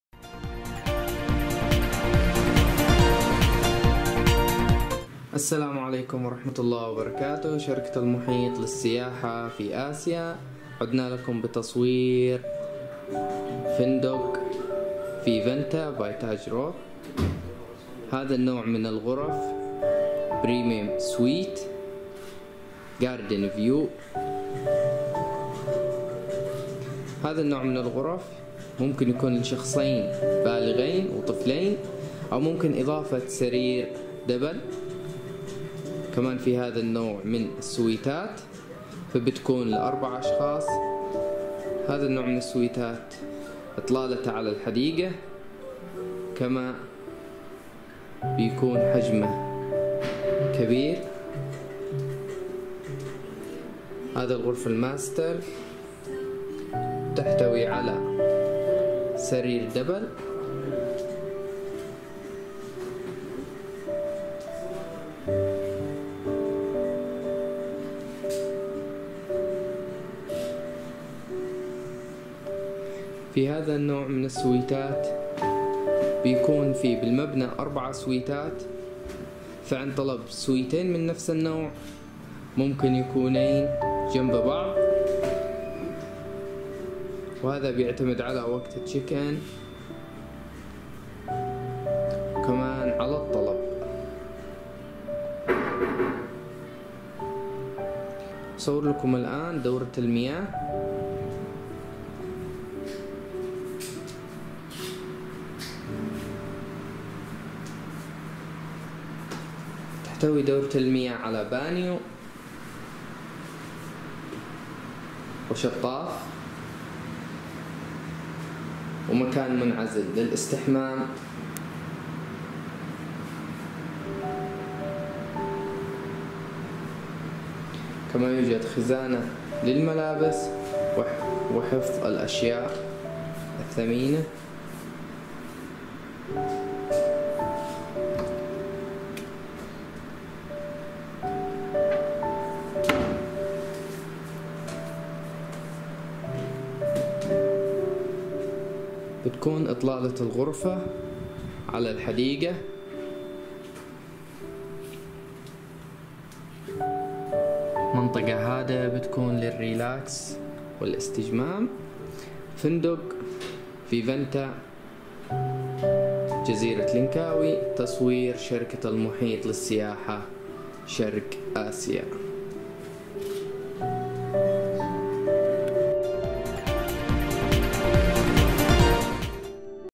فندق فيفانتا باي تاج لنكاوي | غرفة برميم سويت مطلة على الحديقة | Vivanta by taj langkawi
للحجز والاستفسار تواصلوا معنا عن طريق الوتس اب على الرقم ( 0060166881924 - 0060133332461 ) او تواصلوا معنا على الواتس اب عبر الرابط

 فيفانتا باي تاج لنكاوي

يعد فندق فيفانتا باي تاج لنكاوي vivanta rebak island langkawi واحداً من افضل فنادق لنكاوي المناسبة بشكل كبير لقضاء افضل أوقات الهدوء والاسترخاء بعيداً عن ضجيج المدن حيث يقع فندق فيفانتا لنكاوي على جزيرة خاصة في قلب الغابات الخضراء المورقة مع شاطئ خاص به.

 واسم الجزيرة التي يقع فيها منتجع فيفانتا باي تاج لنكاوي هي جزيرة ريباك rebak island احدى الجزر القريبة من لنكاوي كواه ماليزيا ويمكن الوصول اليها عن طريق القوارب السريعة ويقع مطار لنكاوي الدولي على بعد 7.3 كيلومتر عن منتجع فيفانتا لنكاوي  , كما يعد فيفانتا باي تاج ريباك من فنادق لنكاوي خمس نجوم الراقية والفخمة في لنكاوي ماليزيا.